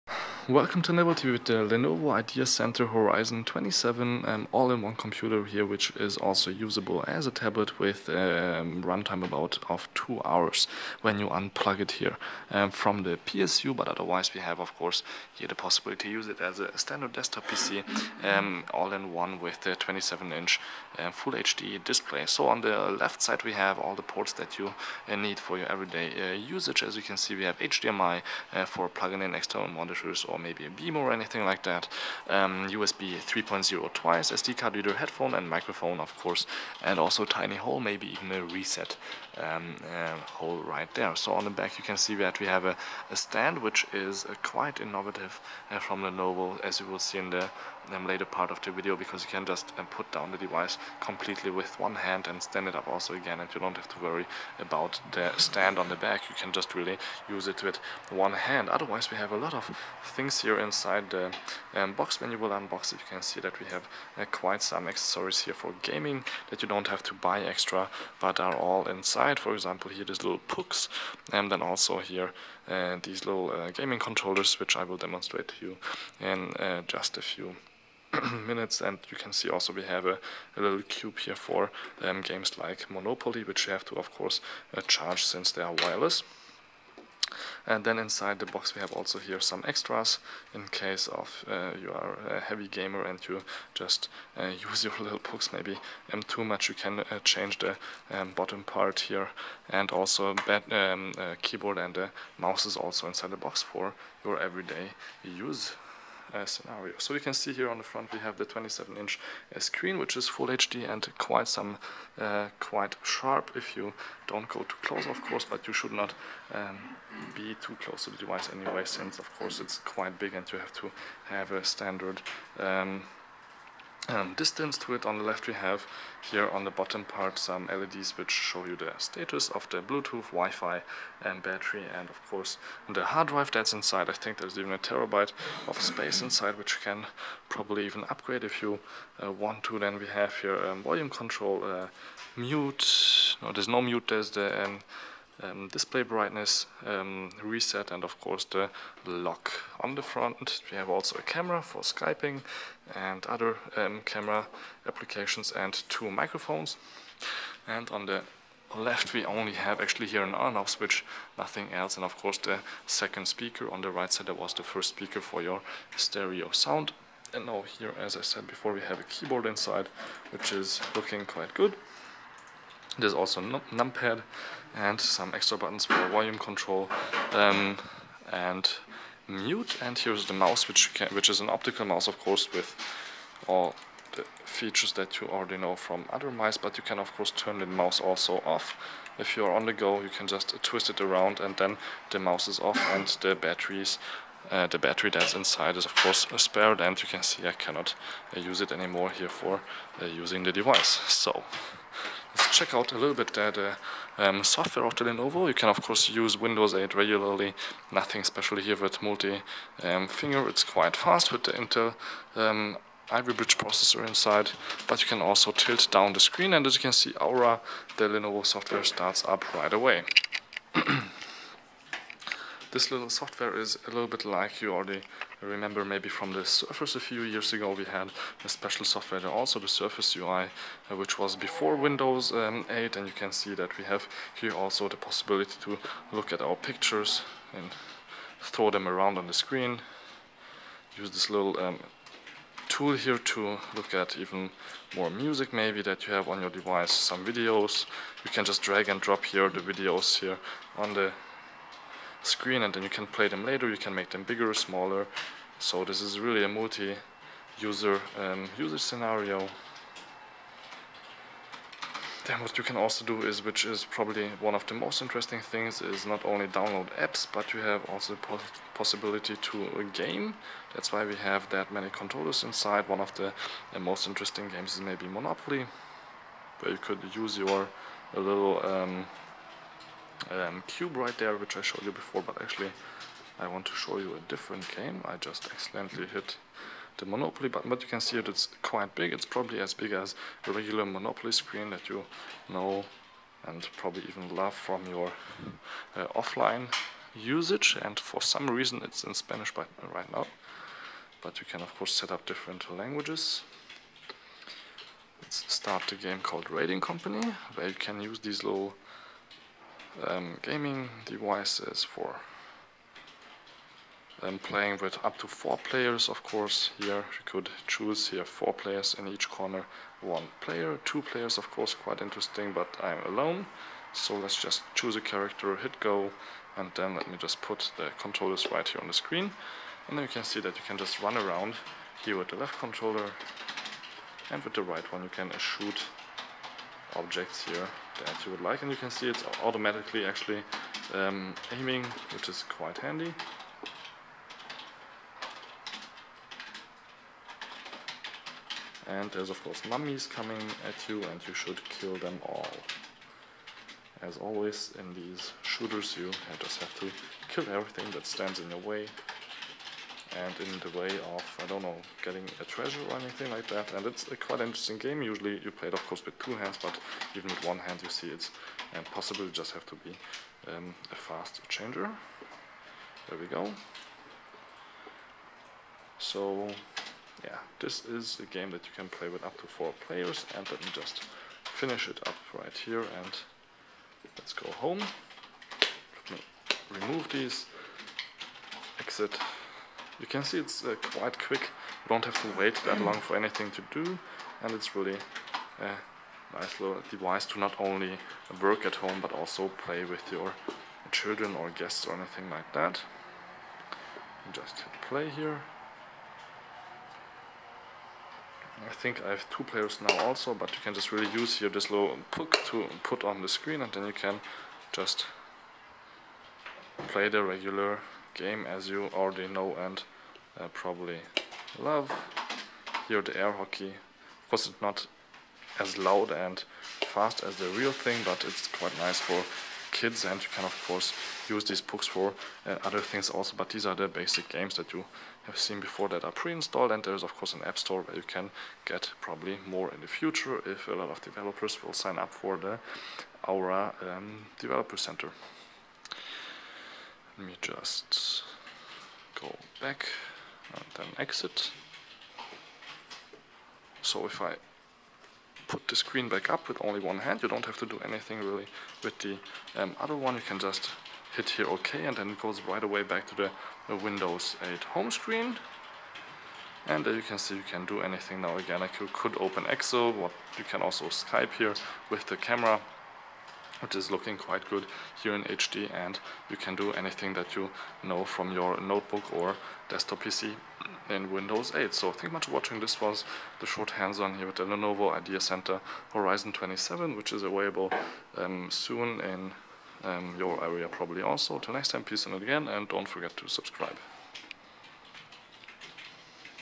Lenovo Horizon 27 Hands On
- Lenovo Horizon 27 All in One Computer in a short Hands On with different Games and all kind of accessories.